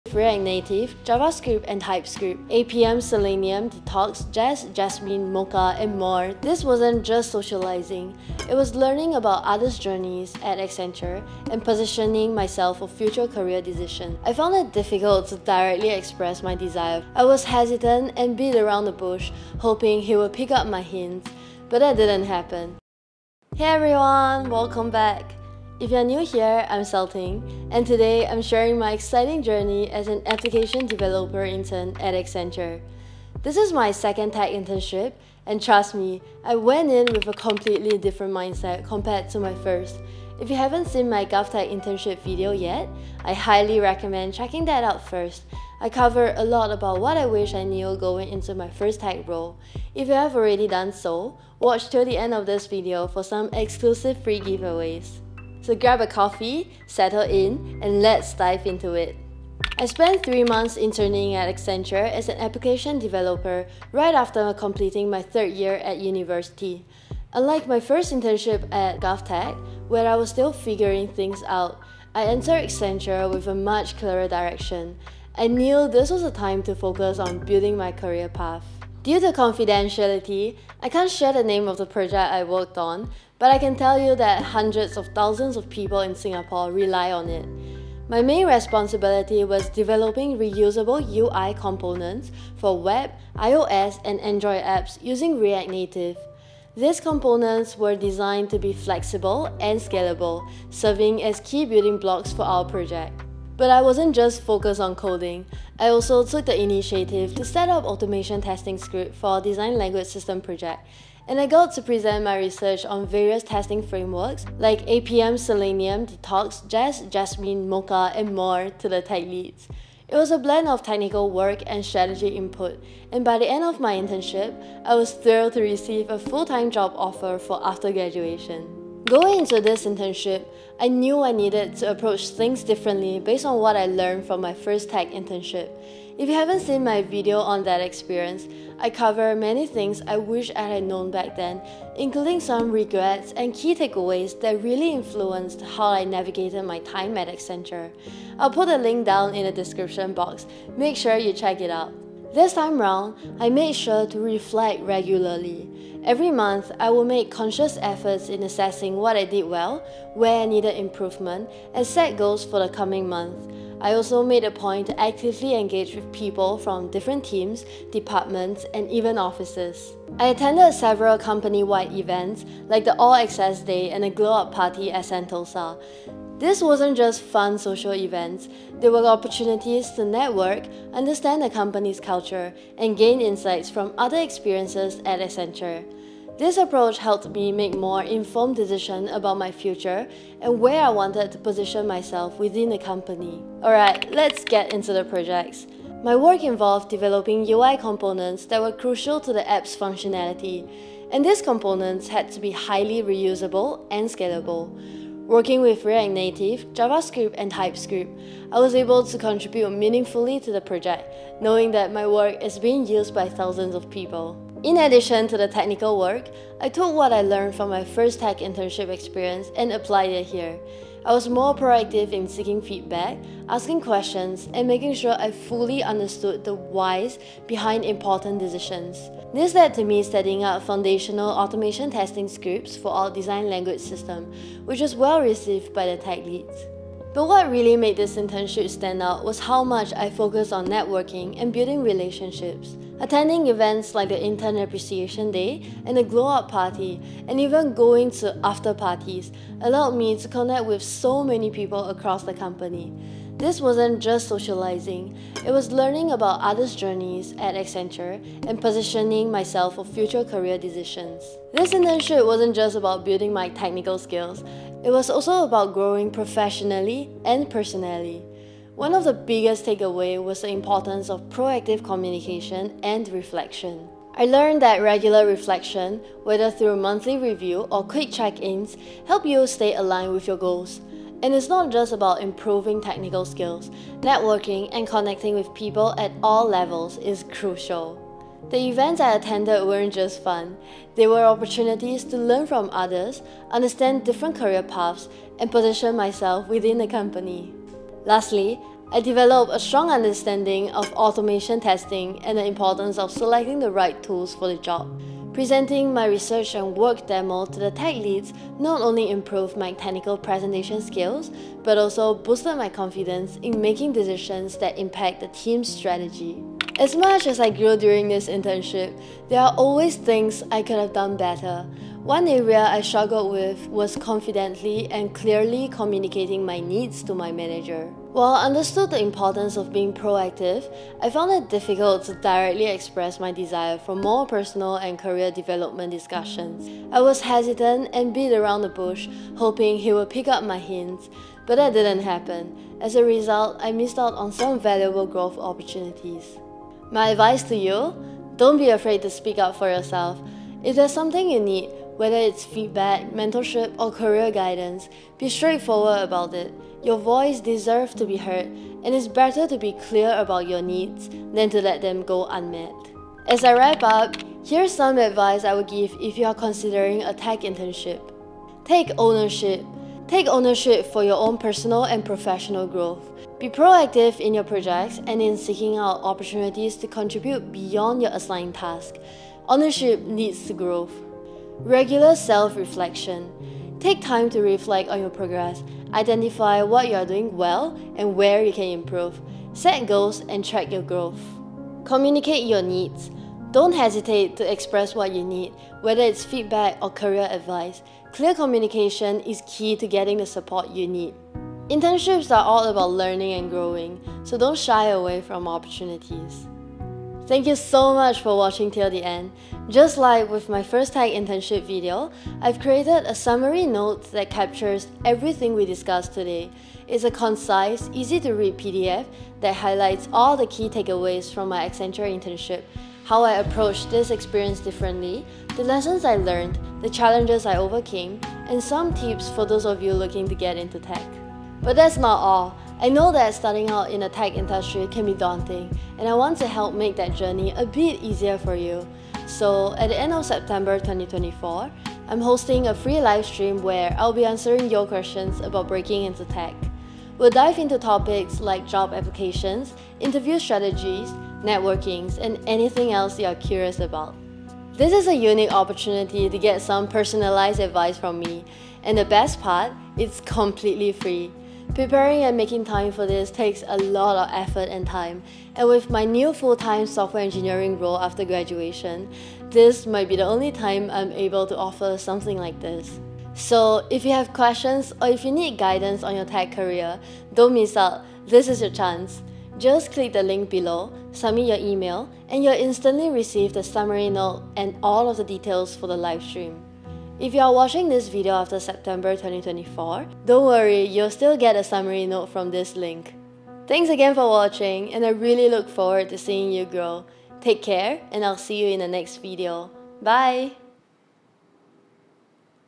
From Challenges to Growth: Inside a Tech Internship | Lessons and Career Tips
🚀 Discover the lessons and career tips learned from a tech internship, from overcoming challenges to personal growth.

✅ Get your one-page summary note (covering everything I talked about in this video)

Let's grow in this tech career journey together~

Curious about what it's like to intern as a Software Engineer? Unsure how to approach your tech career?

In this video, I share my journey from overcoming challenges to achieving growth during my Accenture SWE internship. You'll get an inside look at the key lessons I learned, the obstacles I faced, and the strategies I used to navigate my tech career.

Whether you're preparing for your first tech internship or looking to refine your approach, these career tips will help you make the most of your experience.

Don’t miss out on valuable insights that can shape your path in the tech industry!

Timecodes

0:00 - Overview
0:23 - Introduction
0:59 - Overview of my Accenture internship
2:09 - What I did differently from my first tech internship
4:21 - Key takeaways
5:16 - What I would do differently
6:03 - Advice for future interns
6:47 - Giveaways